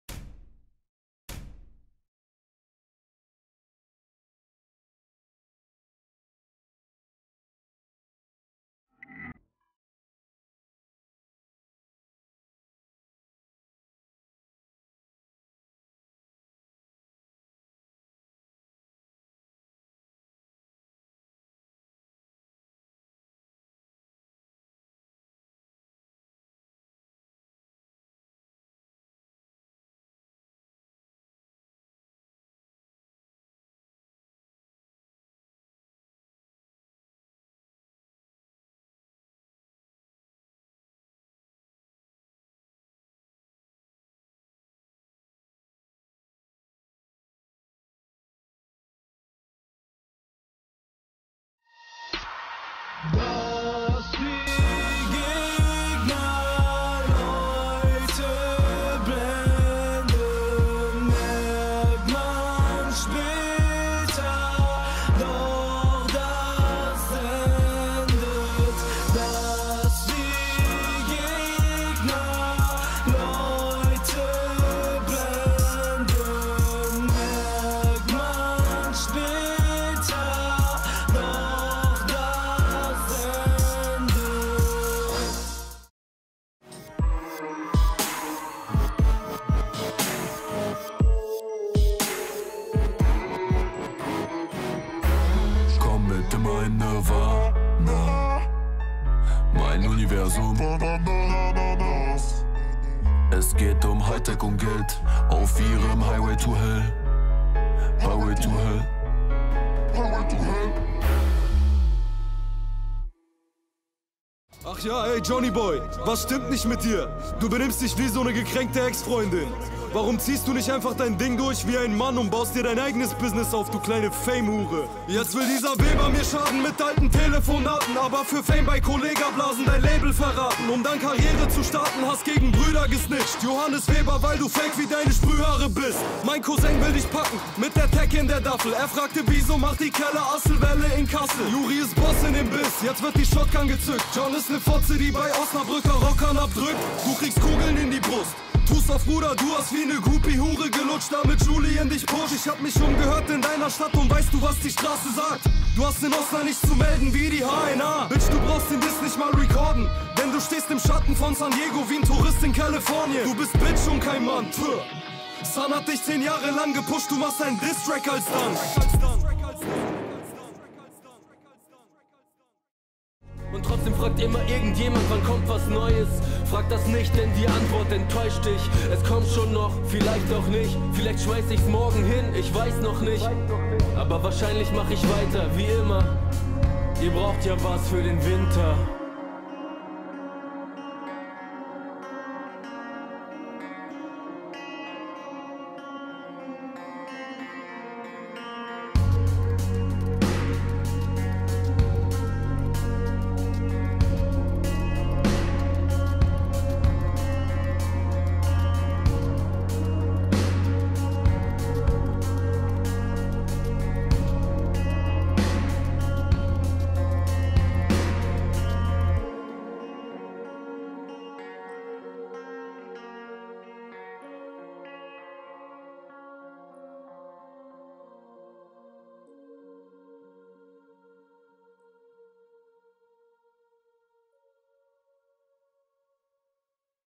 JMC - TOP 5 ATMOSPHÄRISCHE ENDEN
Heute rede ich über die meiner Meinung nach 5 atmosphärischsten Enden in JMC Runden.
Viel Spaß!

Beat im Hintergrund :